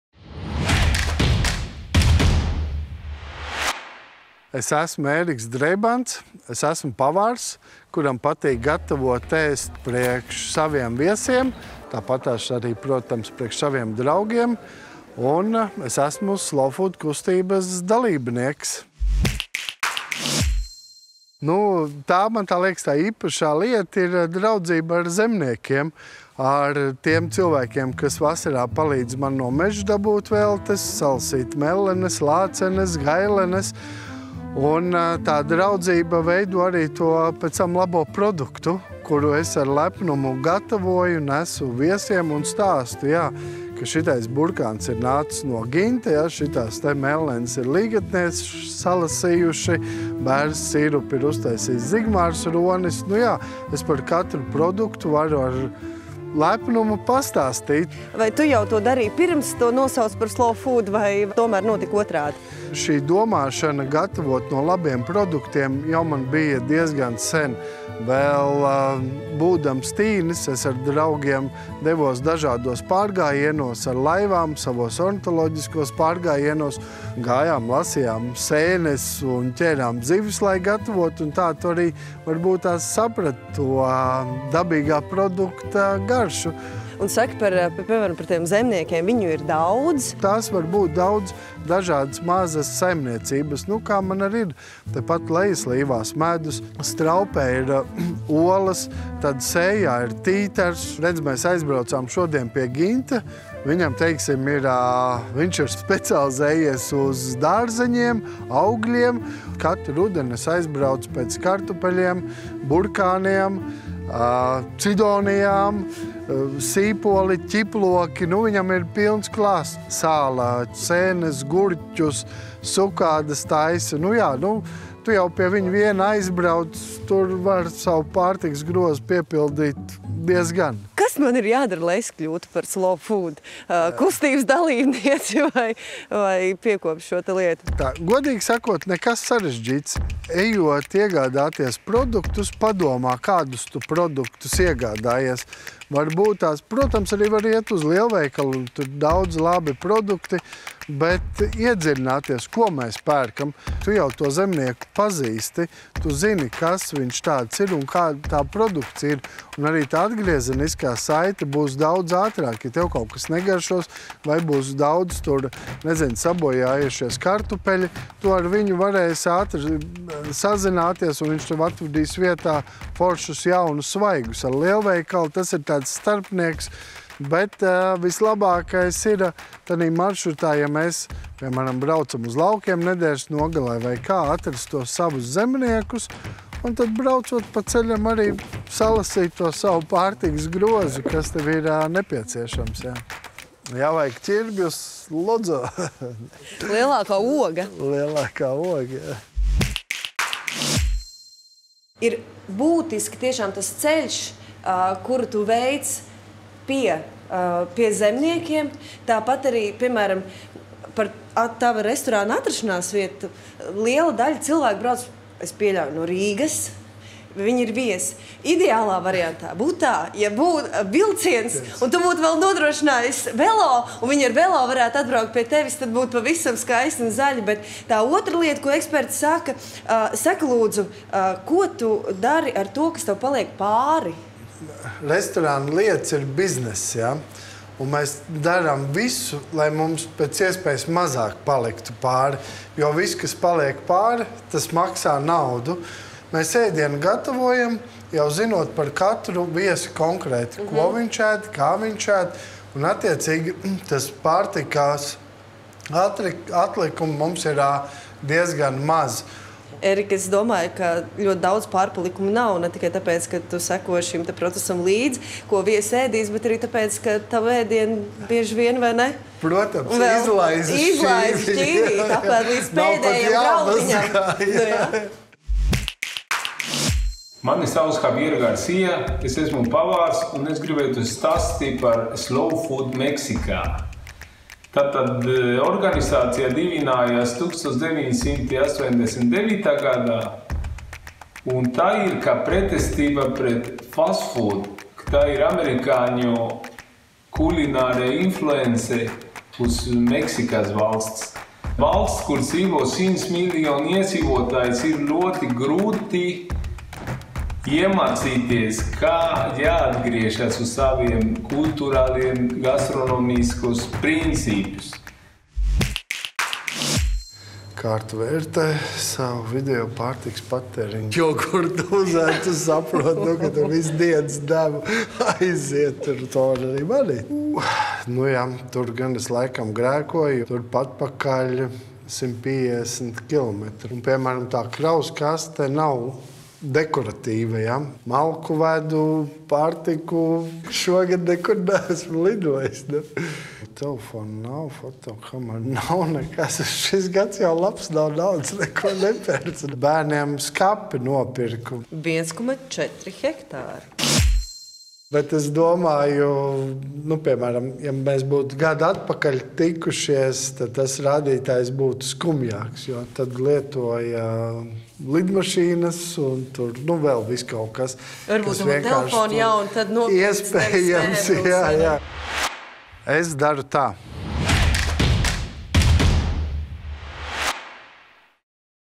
LĒNAIS ĒDIENS / Es daru tā!
Ēriks Dreibants ir pavārs un gan savā darbā, gan ikdienā rūpīgi izvēlas produktus no kādiem gatavot ēdienu. Priekšroka vienmēr tiek dota vietējiem un sezonāliem produktiem.

Projektu “ES DARU TĀ” finansiāli atbalsta Latvijas vides aizsardzības fonds.